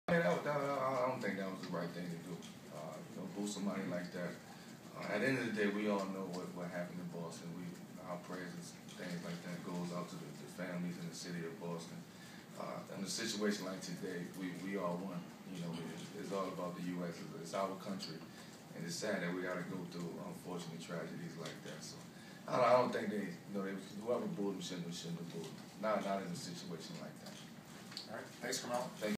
Carmelo Anthony says booing Paul Pierce and Boston was wrong
NEW YORK -- New York Knicks star Carmelo Anthony, who scored 36 points in New York's 85-78 Game 1 win over the Boston Celtics Saturday at Madison Square Garden, says it was wrong of New York fans to boo Paul Pierce before the game as both Anthony and Pierce addressed the crowd to pay tribute to the city of Boston, which was attacked this past week.